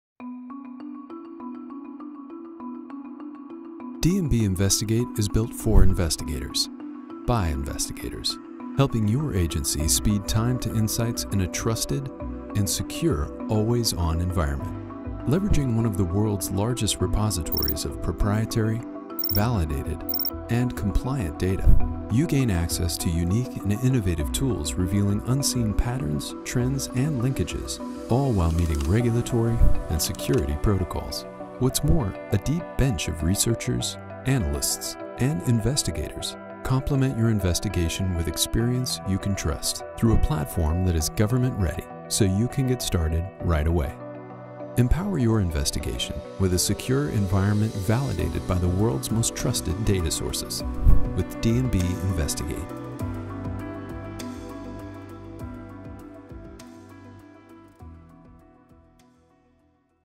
D&B Investigate - Trusted Sources for Government Agencies
D&B Investigate™ - powered by the broadest and deepest set of B2B data and analytical insights available   offers instant access to Dun & Bradstreet’s experienced investigators, auditors,  and researchers to fill any gaps.